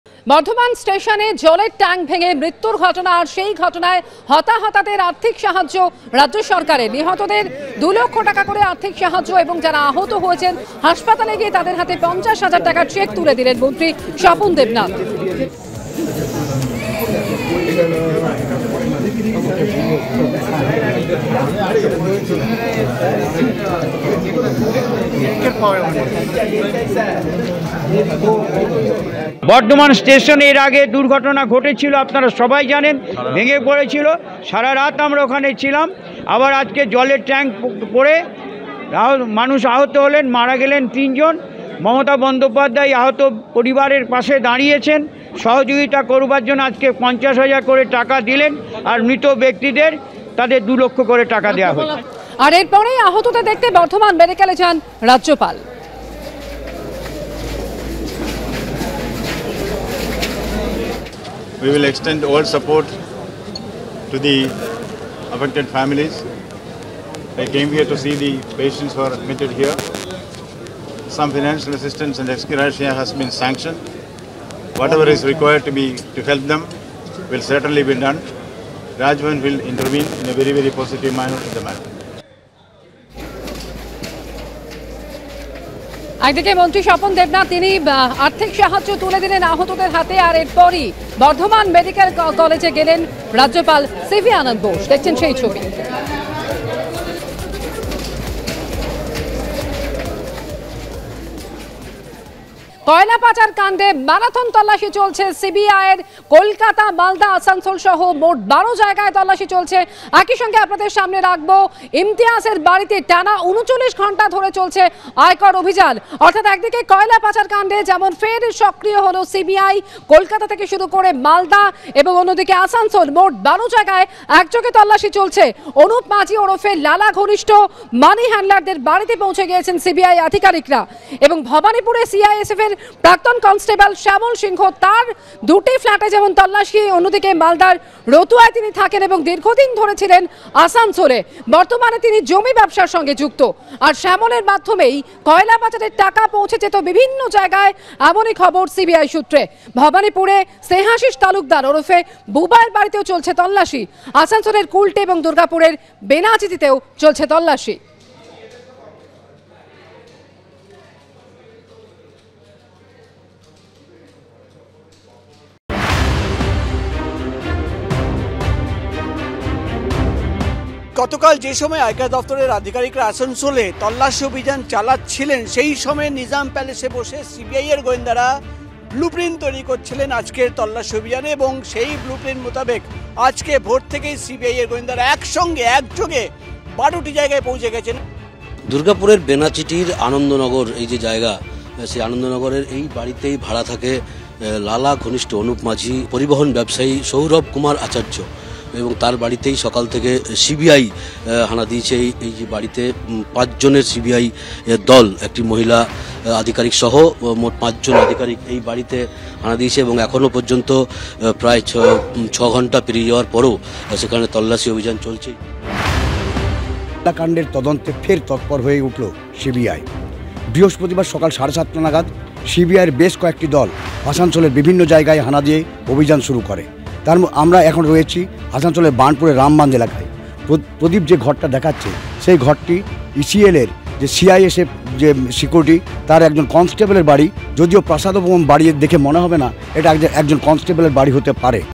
Burdwan Station : বর্ধমান স্টেশনে নিহতদের ২ লক্ষ, আহতদের পরিবারকে ৫০ হাজার অর্থসাহায্য রাজ্যের। ABP
বর্ধমান স্টেশনে হতাহতদের আর্থিক সাহায্য রাজ্য সরকারের। নিহতদের ২ লক্ষ টাকা। আর্থিক সাহায্য। হাসপাতালে গিয়ে আহতদের হাতে ৫০ হাজার টাকা করে চেক তুলে দিলেন মন্ত্রী স্বপন দেবনাথ। এরপরেই আহতদের দেখতে বর্ধমান মেডিক্যালে যান রাজ্যপাল।